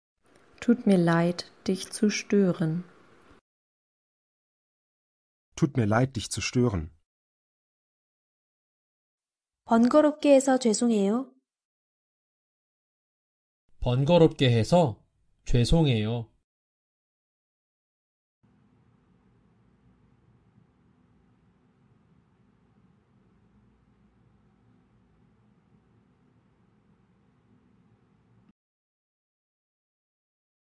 독일어로 "번거롭게 해서 죄송 해요." 말하는 법
남자 목소리 :
Tut mir leid dich zu stören.

남성 음성 번역 :
번거롭게 해서 죄송 해요.